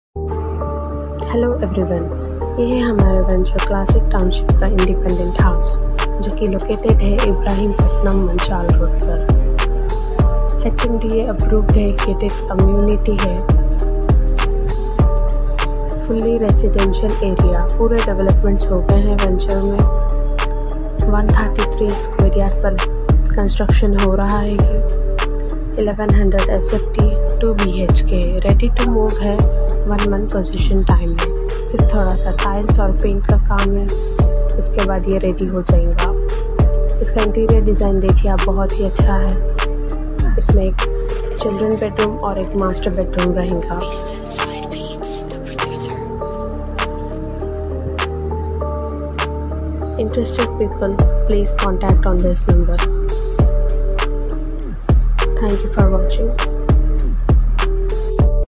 House for sale Hyderabad || Ibrahimpatnam
Ibrahimpatnam @ Manchal Road
Independent house
52 lakhs
North face
111 square yards 1100 sft
30 feet road
Near schools and colleges
As per your requirement